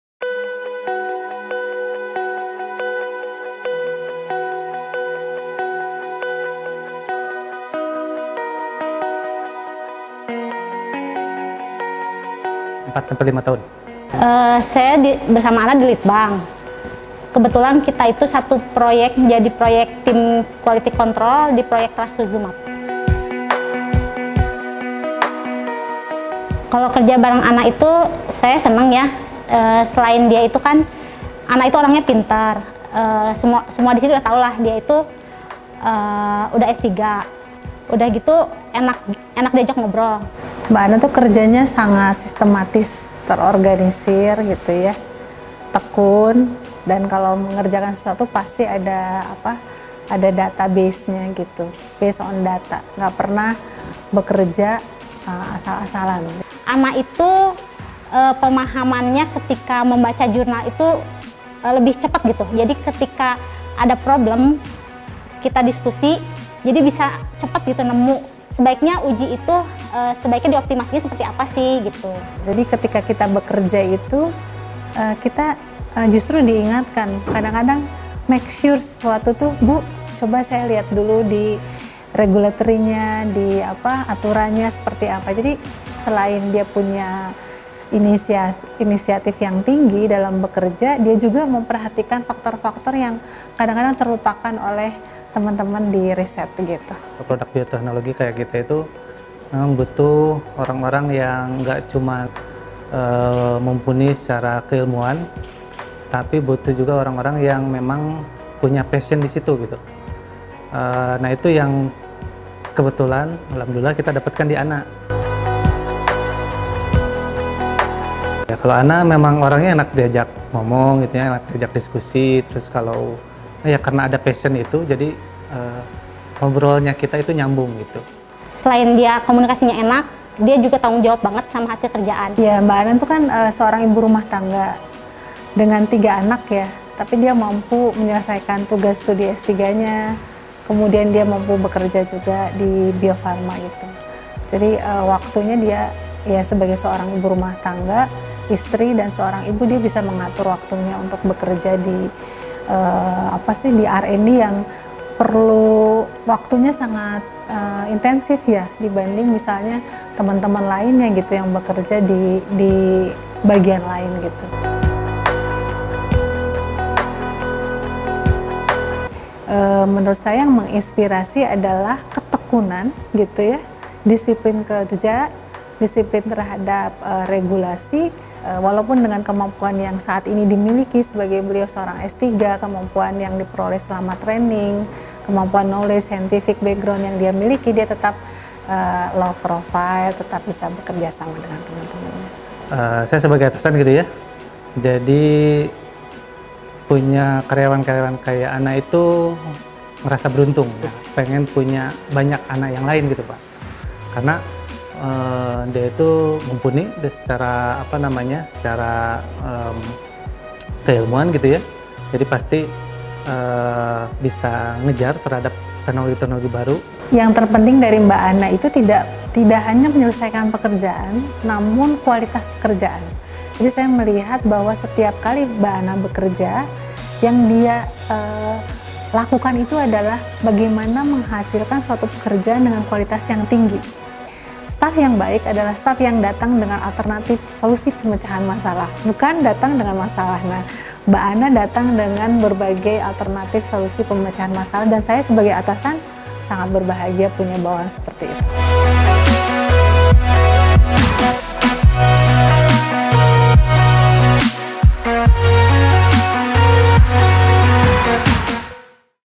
Karyawan Inspiratif Bio Farma - Anna
Inspirasi bisa datang dari mana saja, termasuk dari orang-orang sekitar tempat kita bekerja atau beraktivitas.
Perkenalkan Anna. Ia adalah salah seorang peneliti di Bio Farma sekaligus seorang Istri dan Ibu dari 3 orang anak.